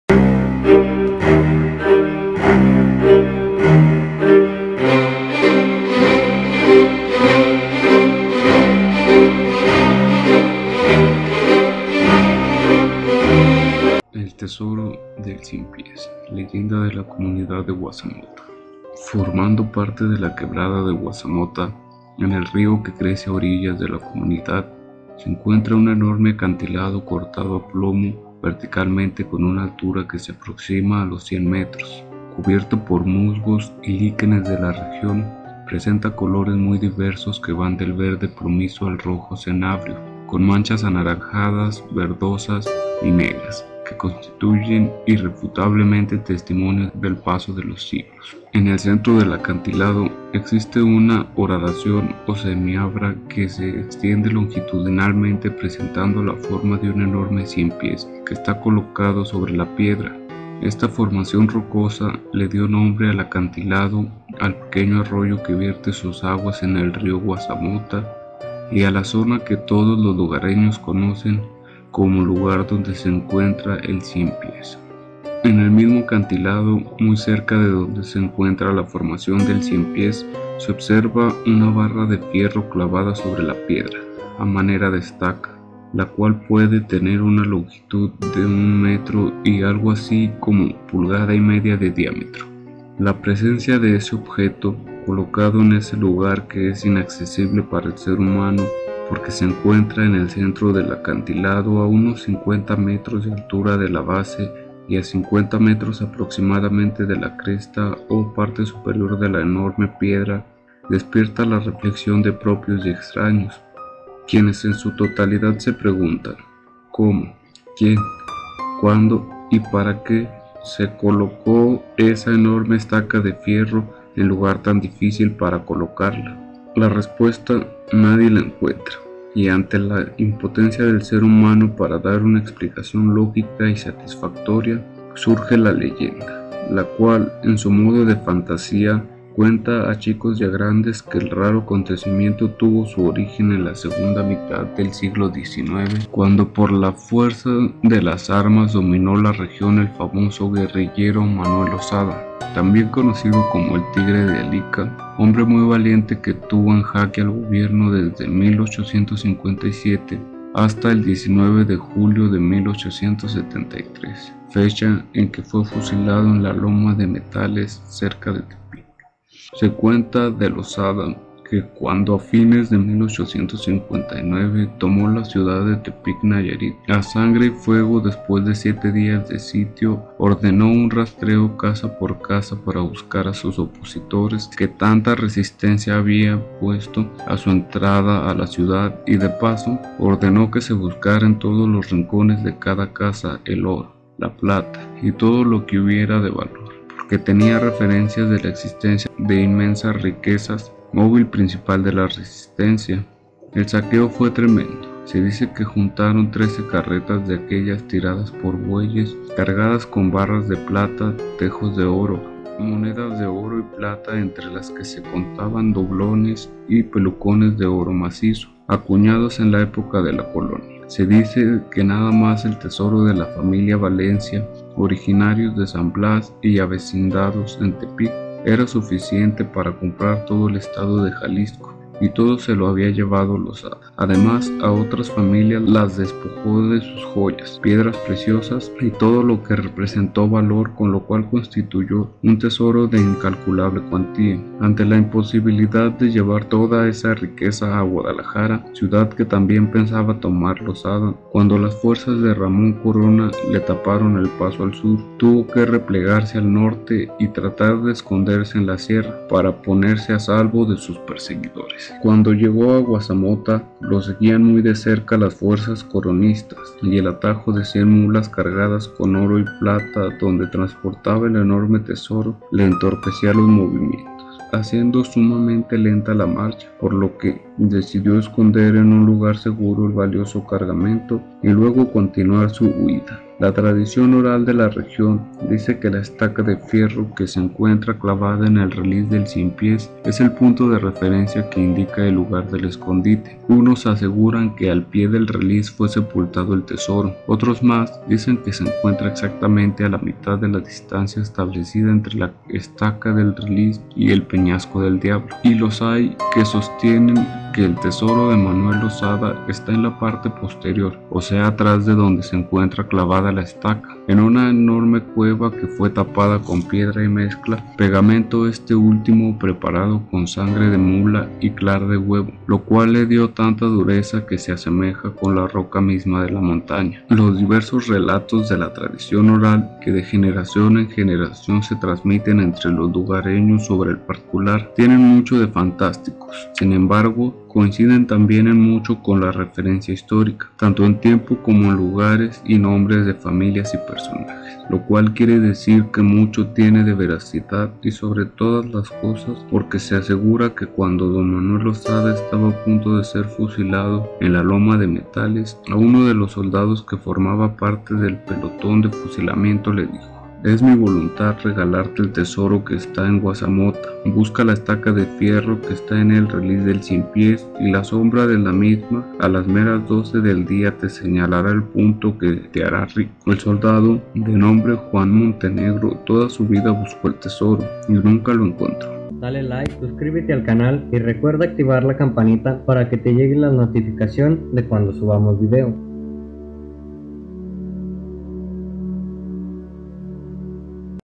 El tesoro del cien pies
A mitad del siglo XIX el guerrillero Manuel Lozada tomo la ciudad de Tepic Nayarit saqueando todo lo que estuviese a su paso acumulando un gran tesoro. Cuenta la leyenda que cuando Lozada iba a ser fusilado por los soldados de El general Ramón Corona le hablo a un soldado del tesoro que había escondido en Huazamota.